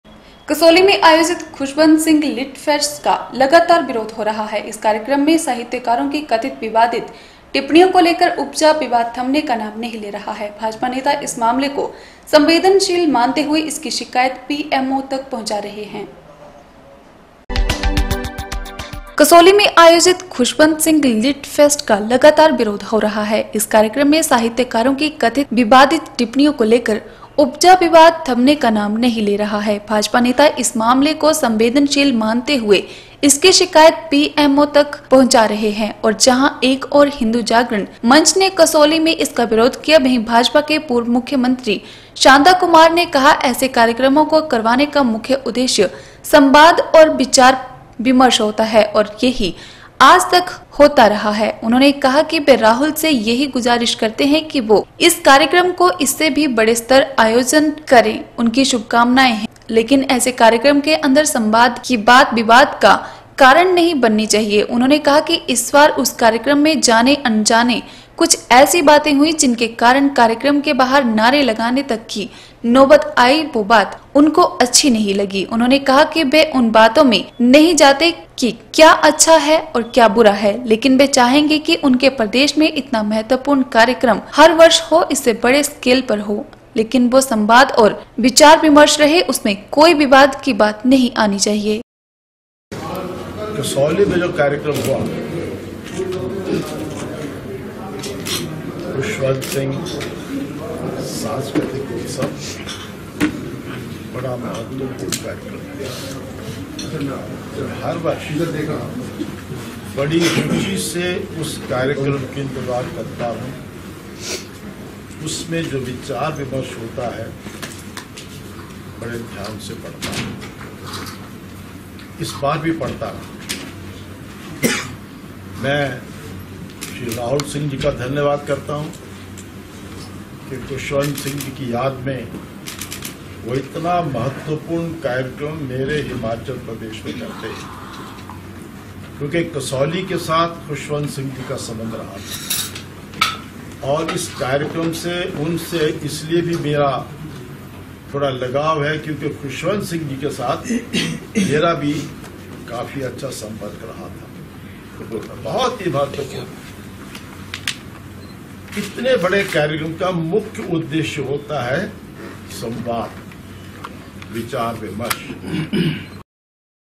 17 OCT N 12 कसौली में आयोजित खुशवंत सिंह लिटफेस्ट का लगातार हो रहा विरोध
कसौली में आयोजित खुशवंत सिंह लिटफेस्ट का लगातार विरोध हो रहा है। इस कार्यक्रम में साहित्यकारों कि कथित विवादित टिप्पणियों को लेकर उपजा विवाद थमने का नाम नहीं ले रहा है भाजपा नेता इस मामले को संवेदनशील मानते हुए इसकी शिकायत पीएमओ तक पहुंचा रहे हैं